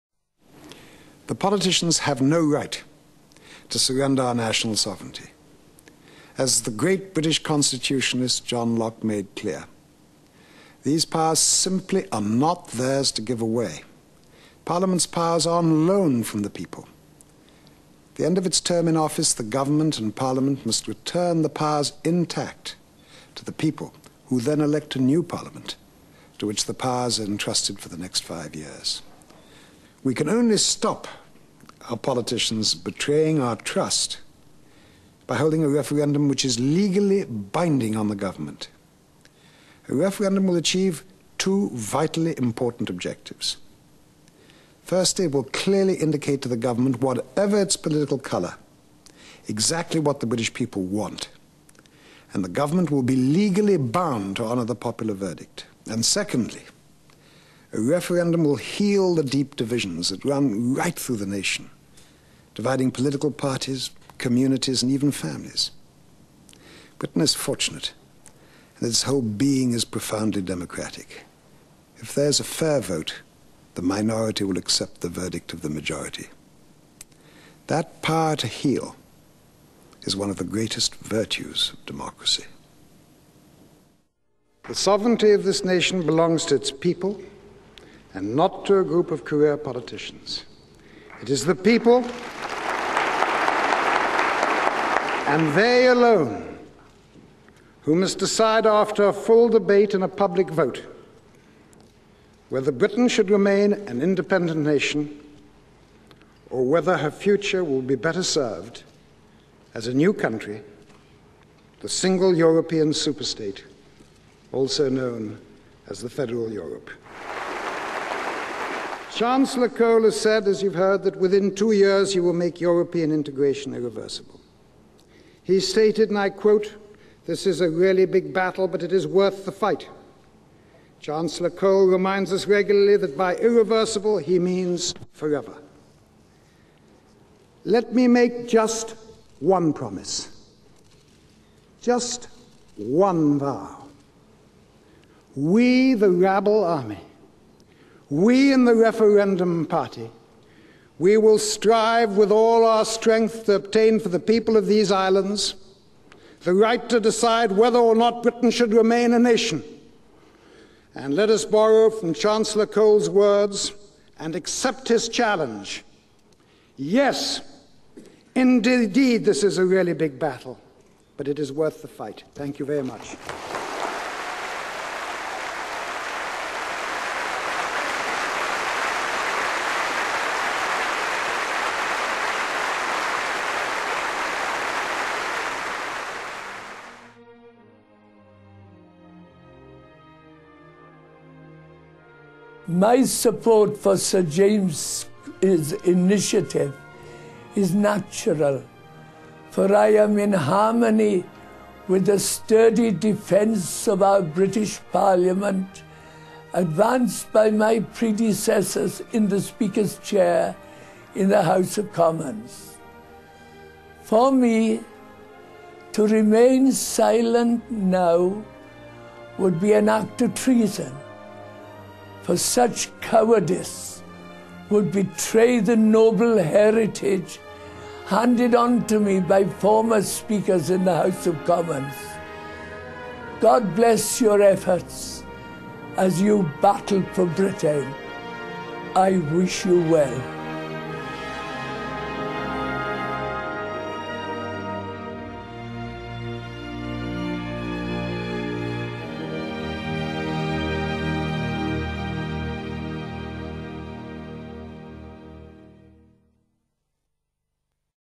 The Most Important Video You'll Ever Watch - Referendum Party 1997 Part 2
A recording of a video sent to 5 million UK homes in 1997. A warning for the UK?